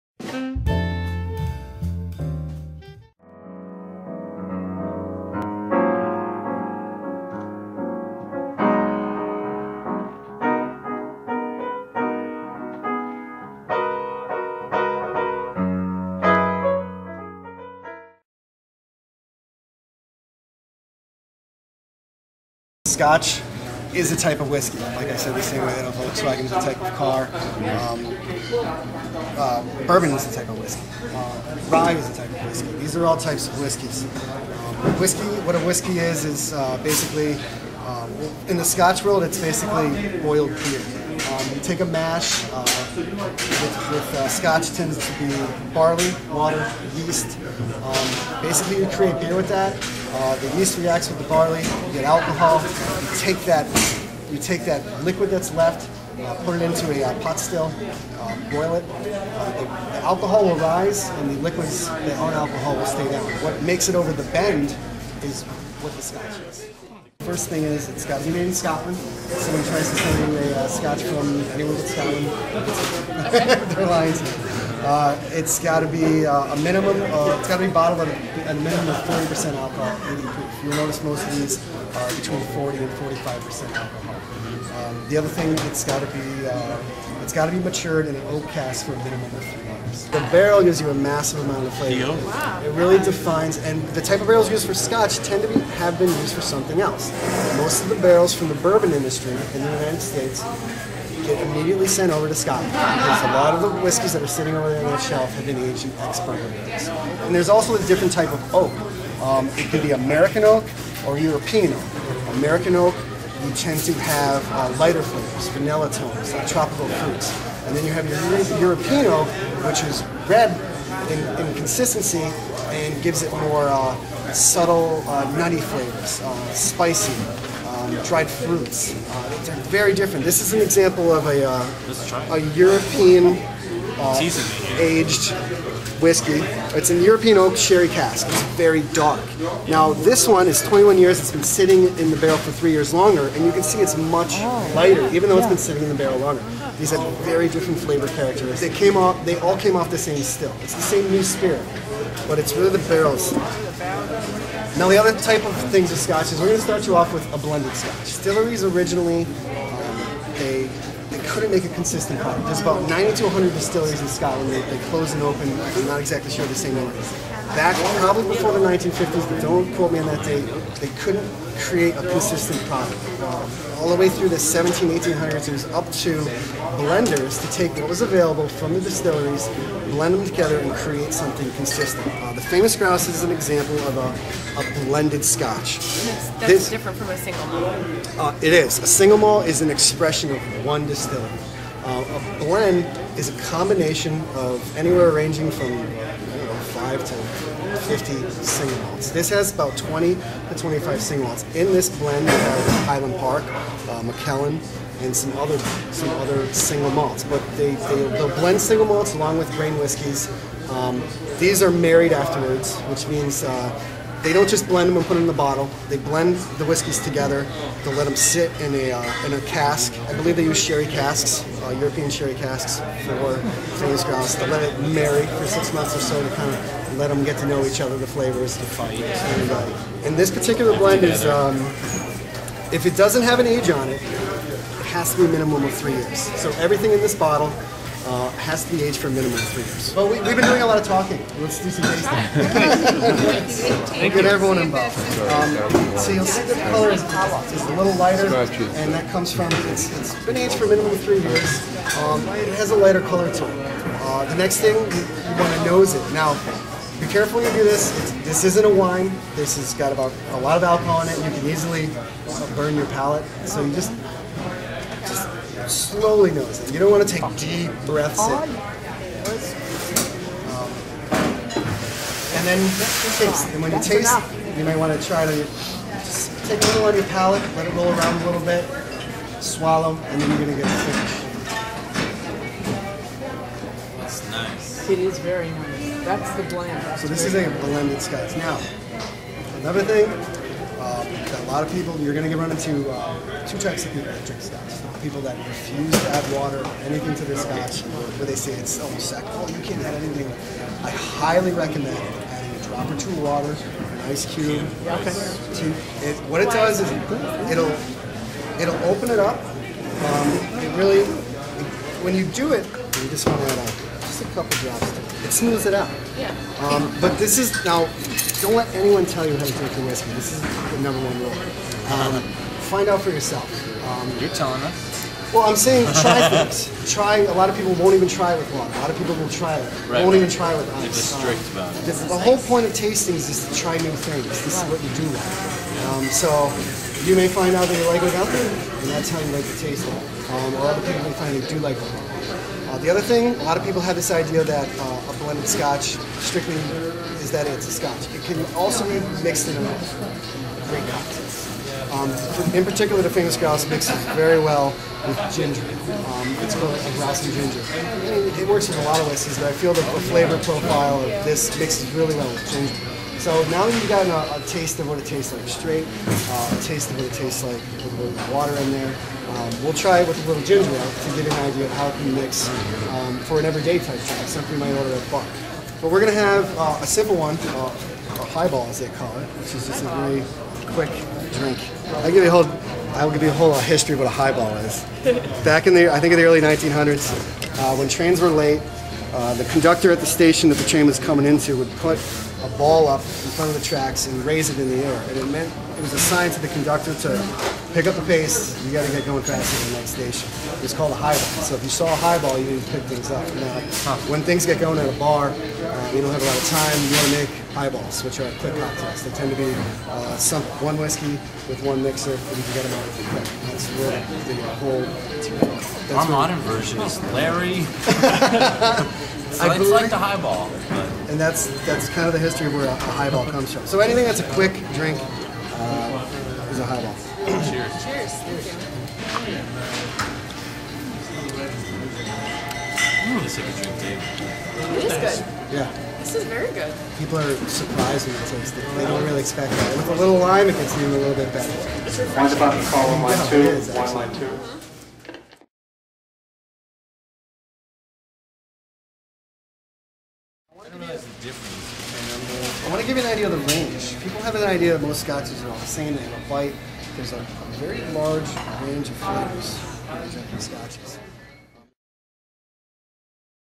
Scotch Whisky Basics at Marty's Liquors
Scotch...what is it? Well first off, it's not as mysterious as some people make it out to be...it's just whisky that is brewed and aged according to a few long standing...well...Scottish...rules. In this segment at Marty's Liquors in Newton, Justin Canofaro of Famous Grouse Scotch takes us through these rules, the tasting process (pay attention here!) and an excellent example of Scotch. This is only part one of the segment. In part two (coming soon), we sample quite a few different types of Scotch and learn about how much flavors can vary from one to another.